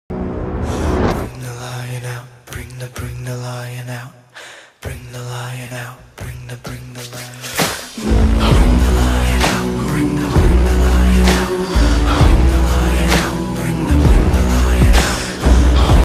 Dominant Lion, sound added, Summon Magical Beasts 1, Paint by Number
Colored this dominant winged lion in the Summon Magical Beasts Collection in the Paint by Number app.  Added a sound clip, check out other video for full image.